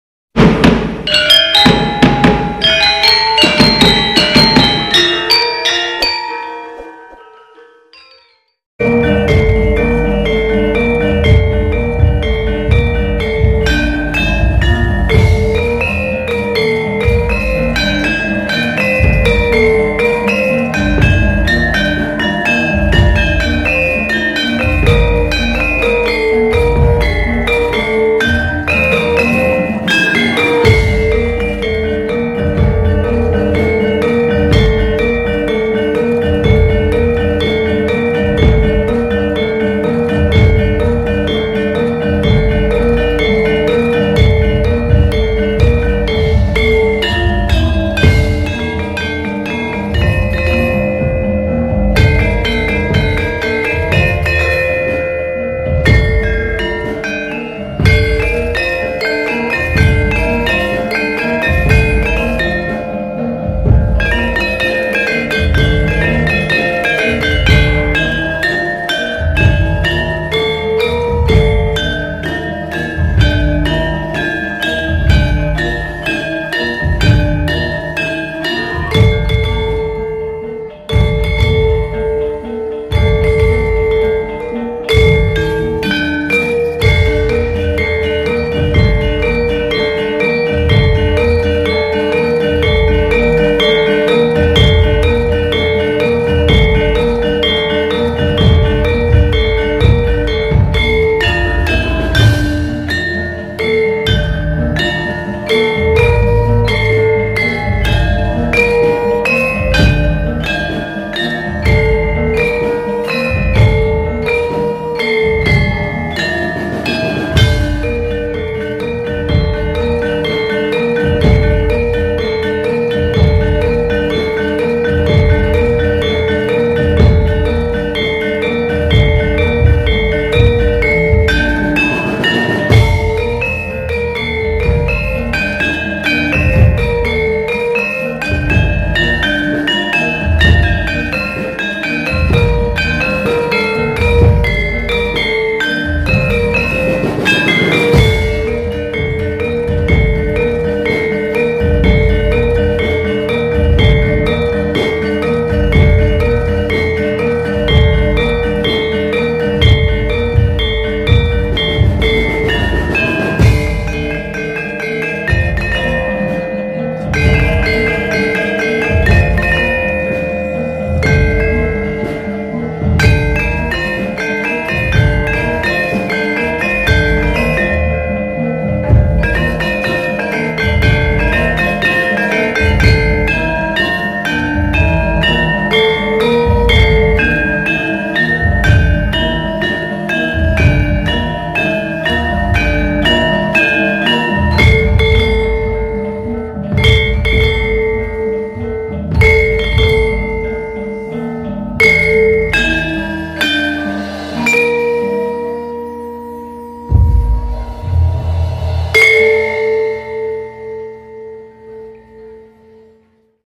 Kekompakan Pengrawit Muda Manduro Ponorogo !! Pentas Wayangan Rutinan Pepadi Ponorogo
Pengrawit Muda Manduro Ponorogo !! Pentas Rutinan Pepadi Ponorogo. Manduro adalah wadah berkumpulnya para seniman muda wayang kulit di Kabupaten Ponorogo.